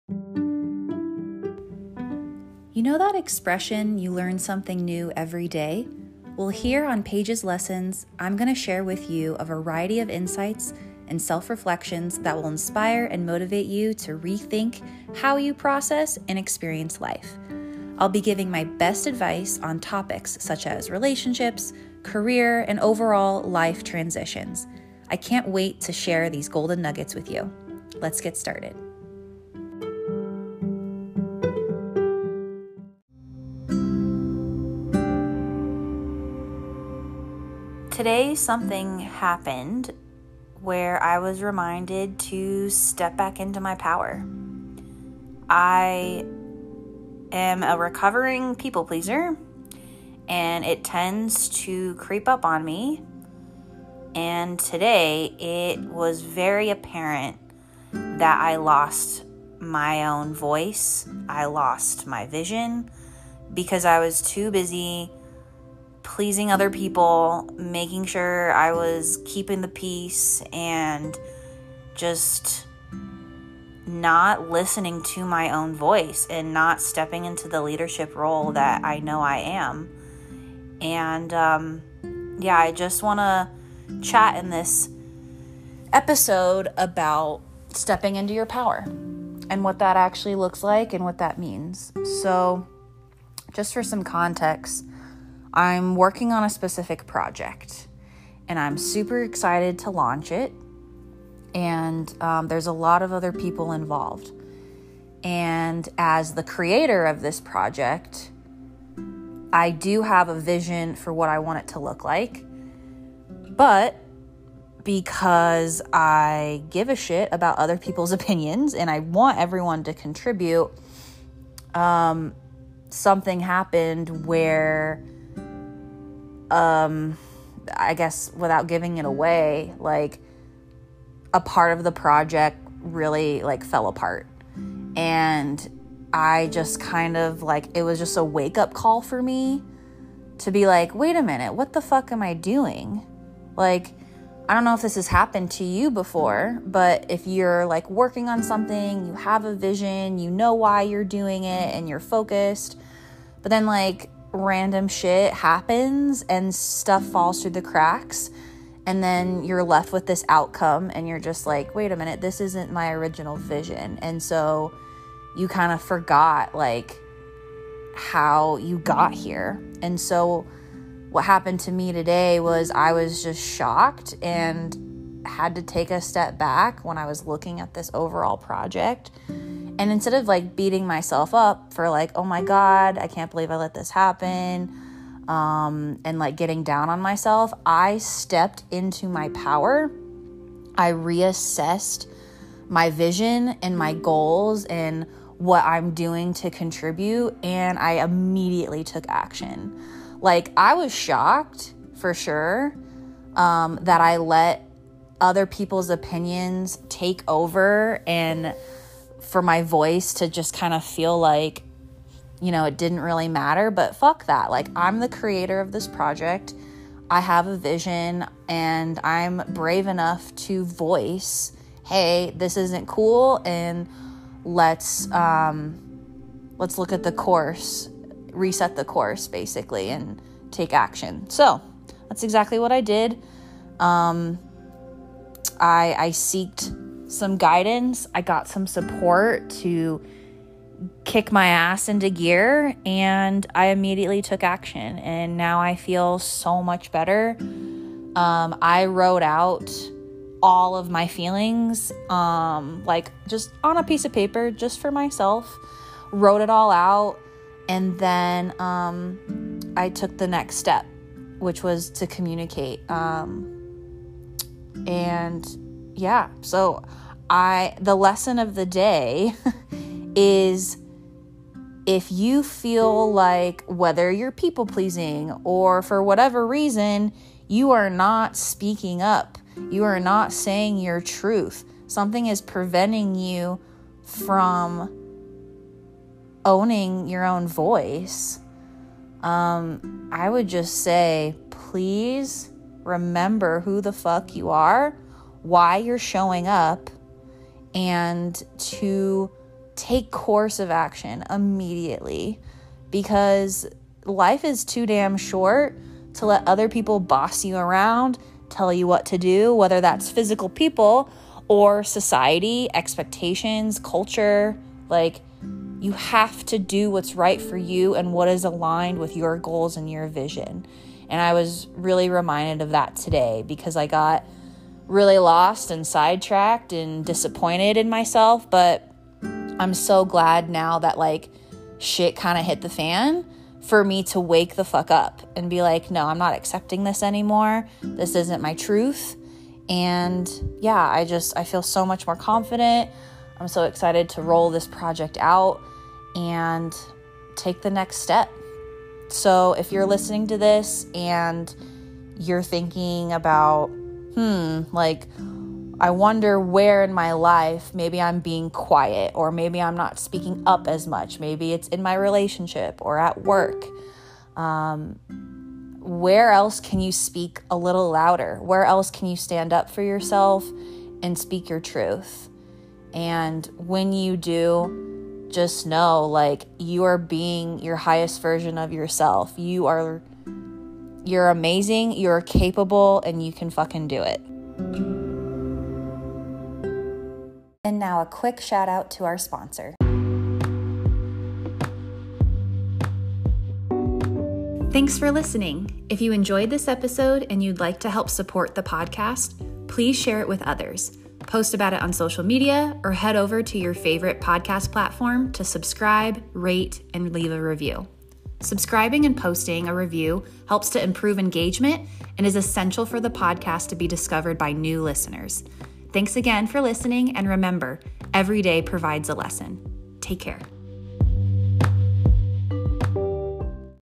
Episode 12: Step into your power!
I was reminded today to use my voice! Don’t get lost in the crowd and stick to your vision.